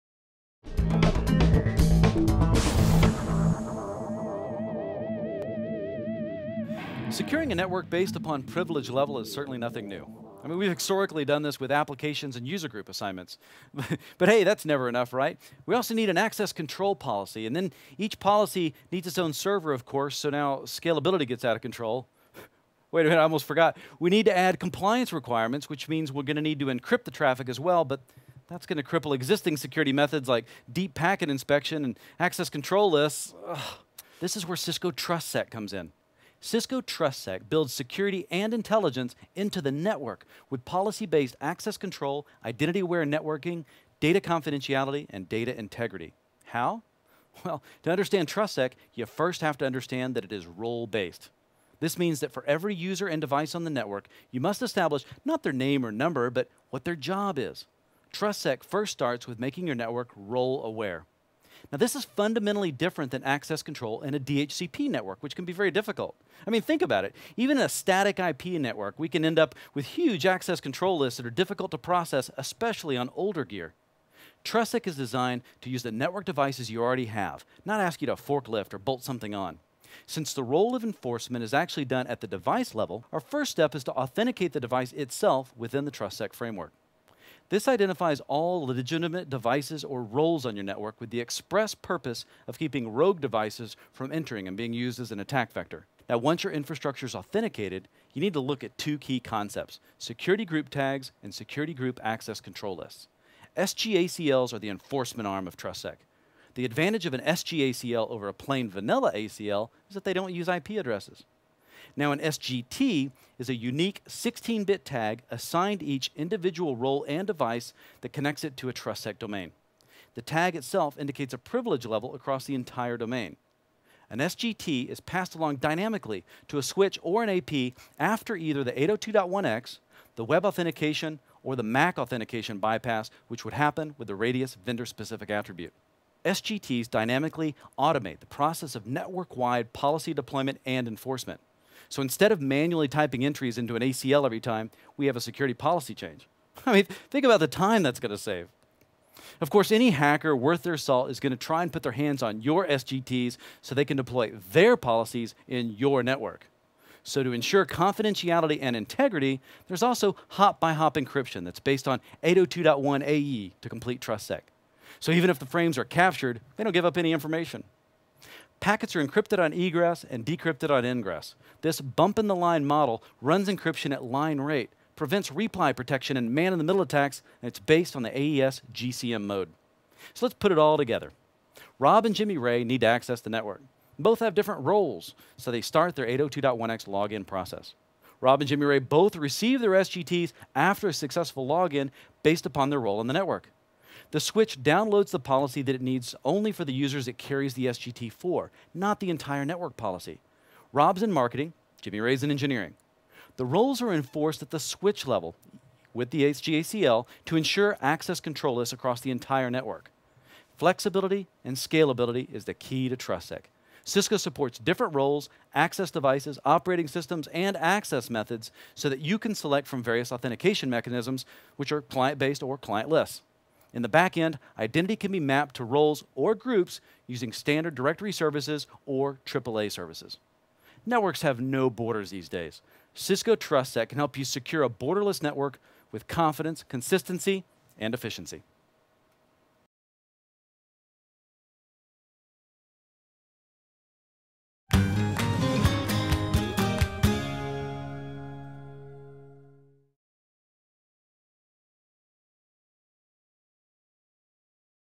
Cisco TrustSec Fundamentals
Scopri come Cisco TrustSec porta la sicurezza e l'intelligenza direttamente nella rete mediante un controllo accessi basato su policy, un networking basato sull'identità, confidenzialità e integrità dei dati e contribuisce a mettere in sicurezza le reti senza confini con tranquillità, coerenza ed efficienza.